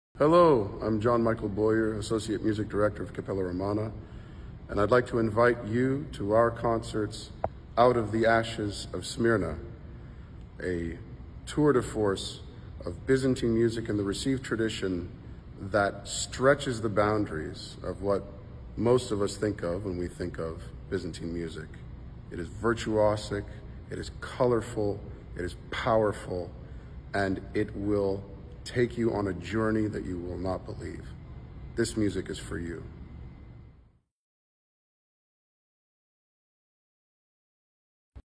John Michael Boyer Invites you to Out of the Ashes of Smyrna
💌 John Michael Boyer offers a personal invitation for you to join us this weekend in Seattle and Portland for OutOfTheAshesOfSmyrna. This music is for you